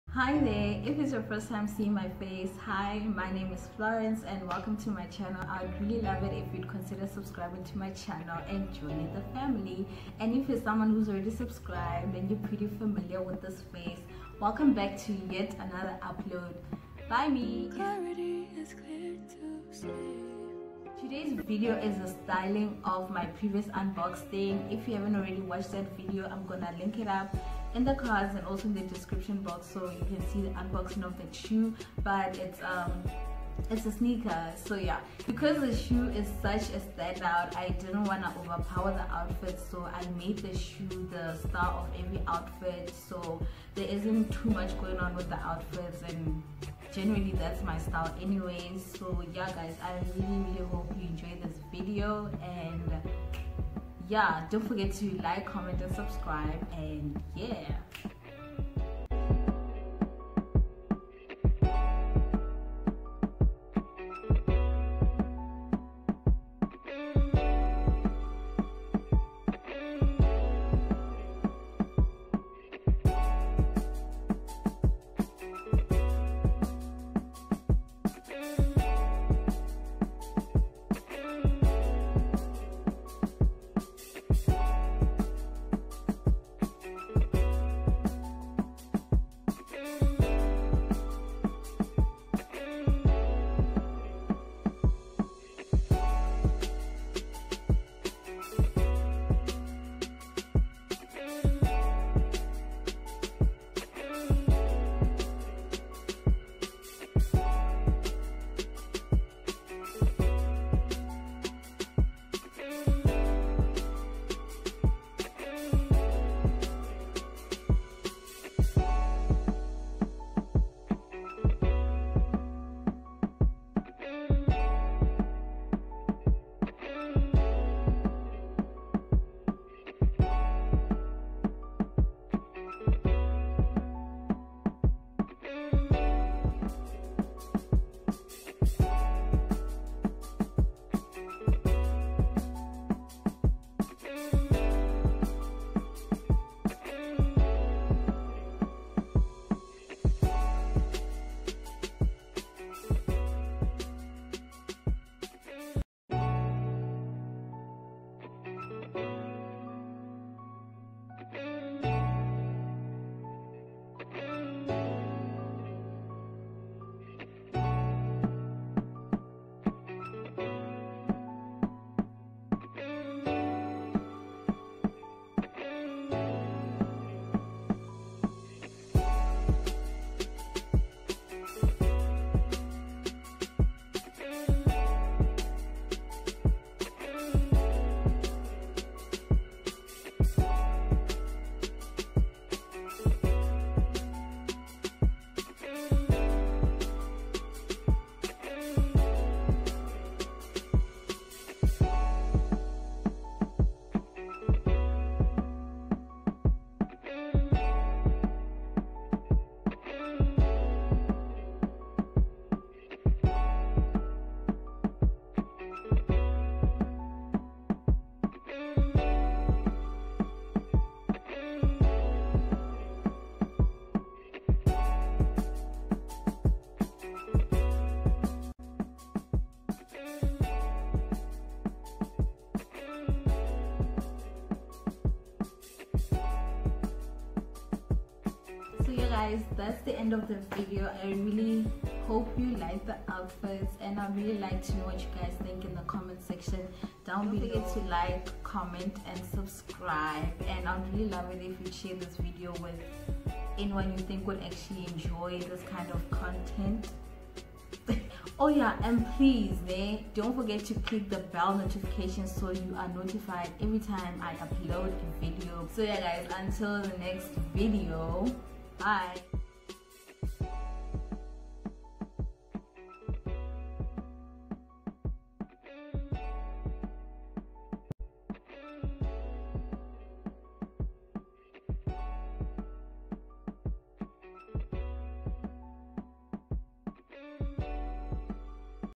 STYLING AIR JORDAN 1 HIGH OG ROYAL TOE || SOUTH AFRICAN YOUTUBER
Hey there😊
This is a short video of me styling Air Jordan 1 Royal toe. When it comes to sneakers I like keeping the outfits simple and letting the shoe stand out, I hope you enjoy it

If you haven't seen my unboxing please check it out I've linked it for you here: